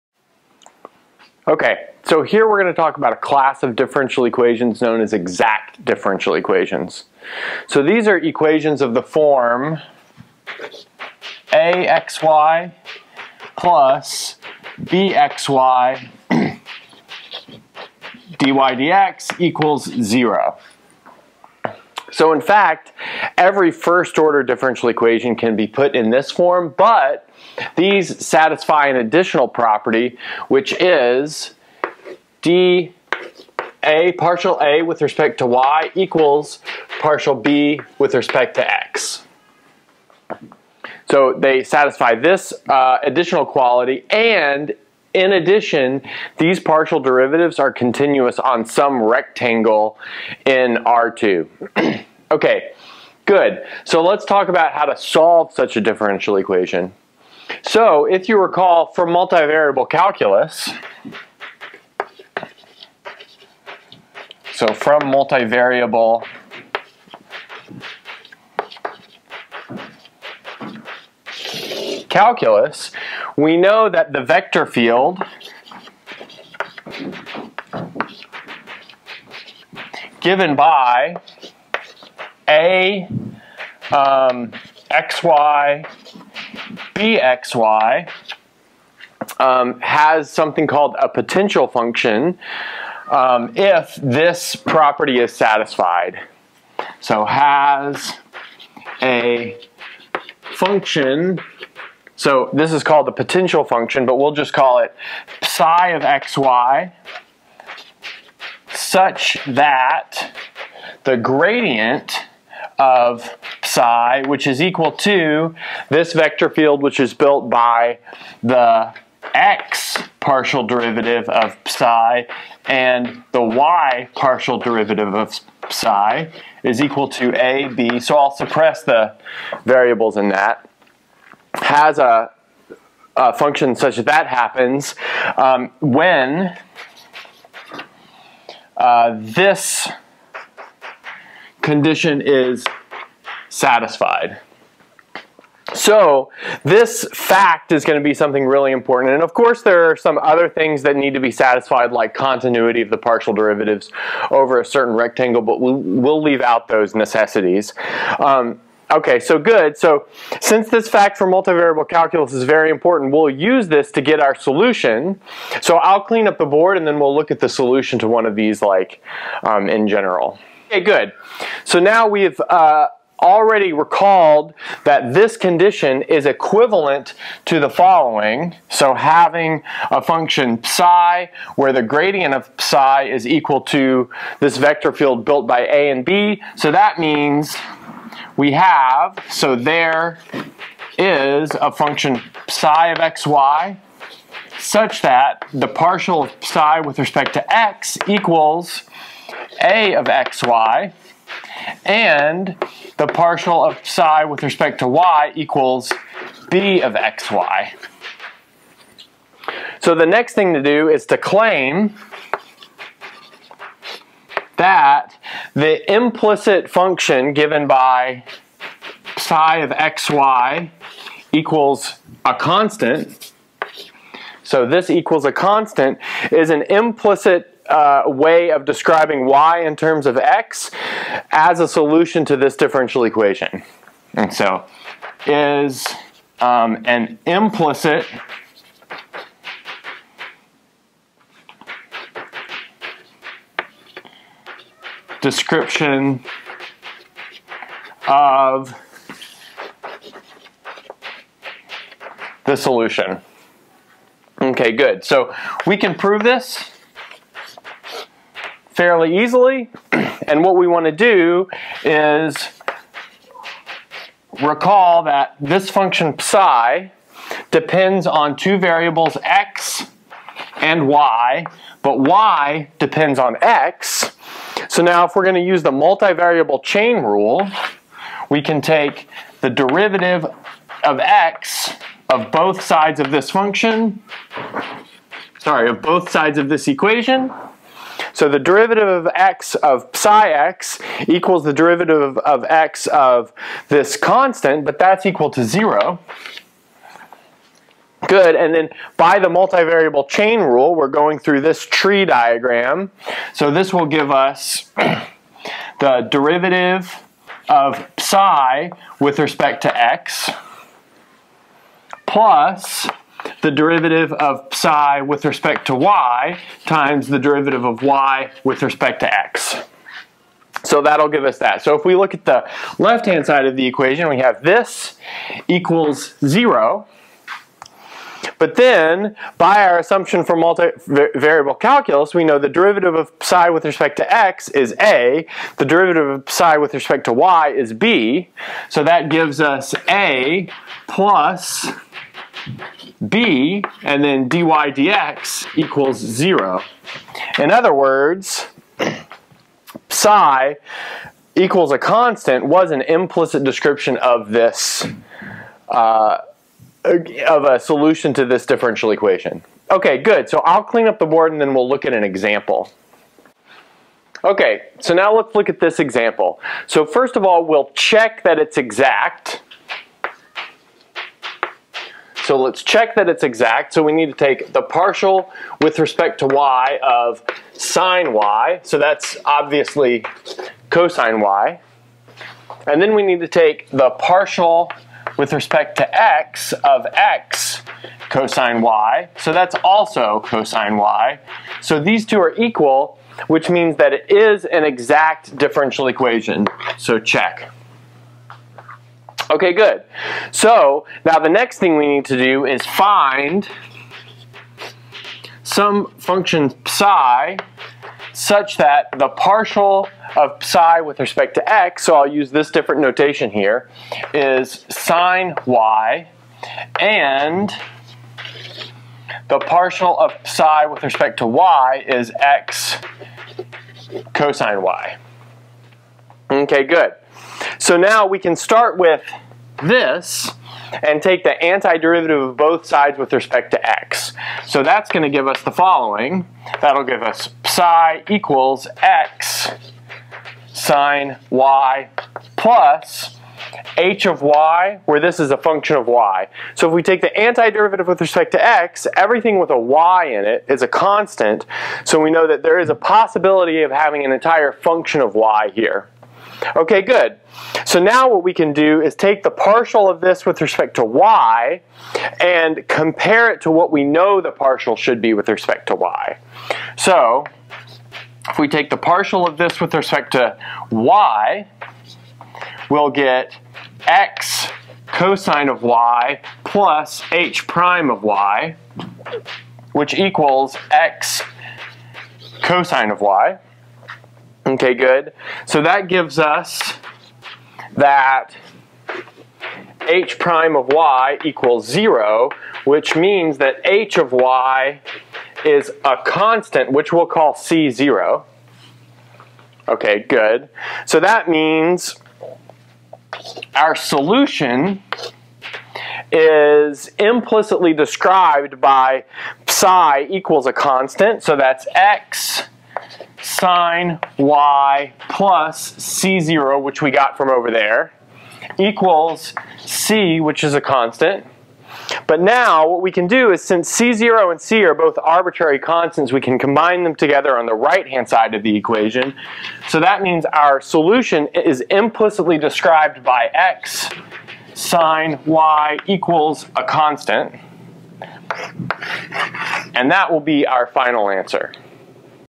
Differential Equations | Exact Equations
We derive the method for solving exact differential equations and give an example.